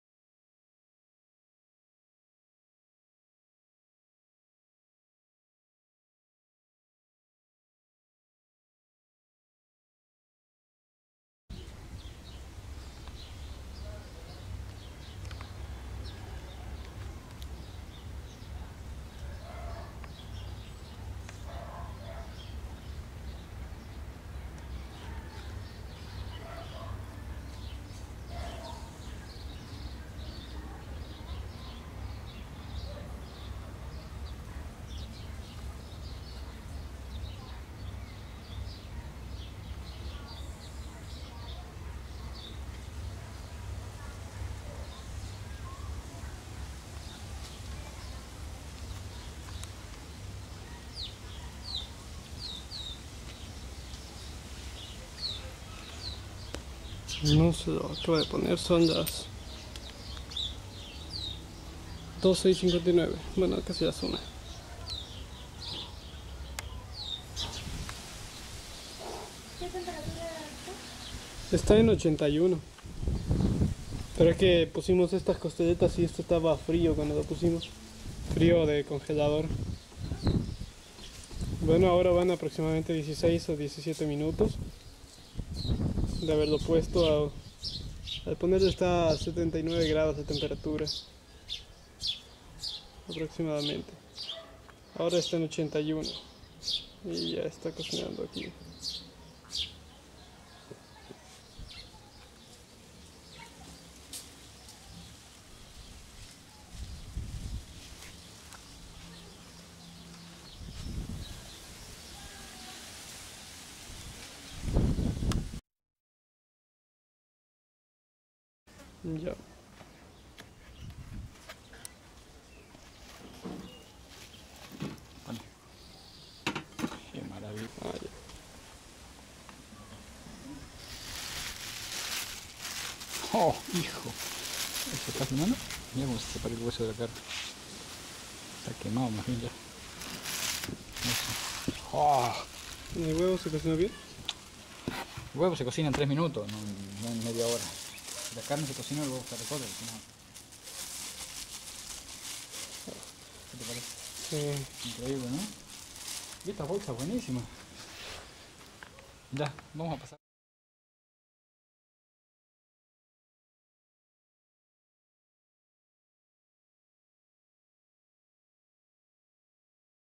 Solar Cooking - Steaks and Egg
Testing cooking some steak and an egg on a recently made solar oven. The meat was taken from the fridge and into the oven. At the end it was well cooked.